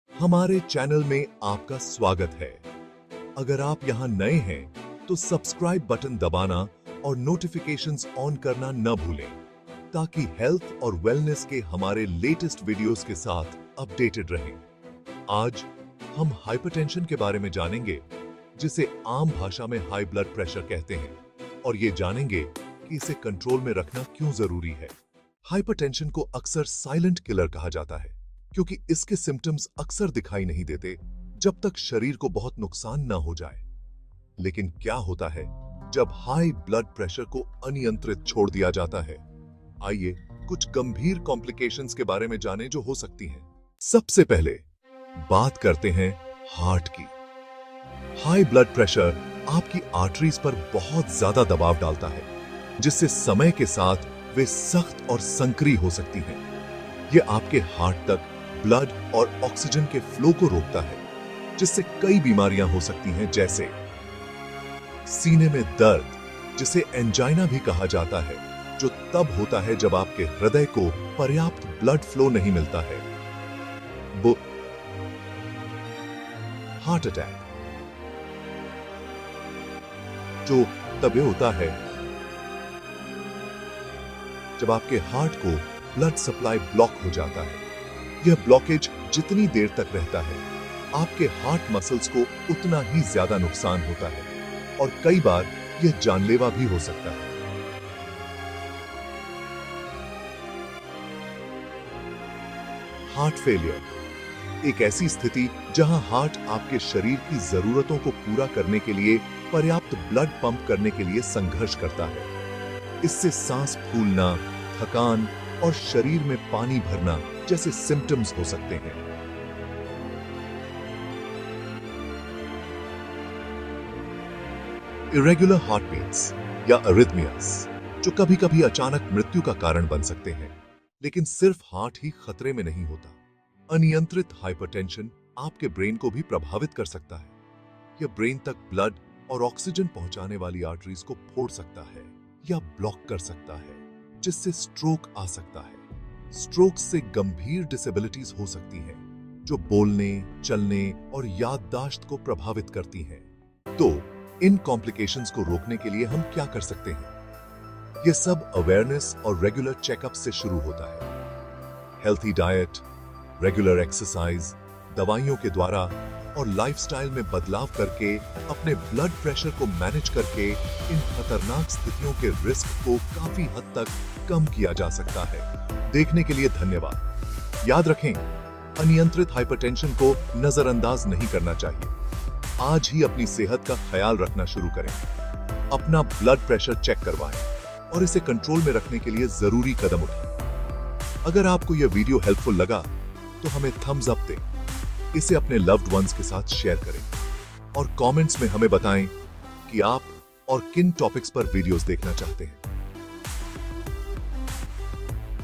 (अनियंत्रित उच्च रक्तचाप )Uncontrolled Hypertension: Dangers and Complications
High blood pressure, or hypertension, can silently damage your body for years before symptoms develop. In this video, we'll explore the serious complications of uncontrolled hypertension, including heart disease, stroke, and more. Learn how high blood pressure can affect your heart and brain and the importance of keeping it under control.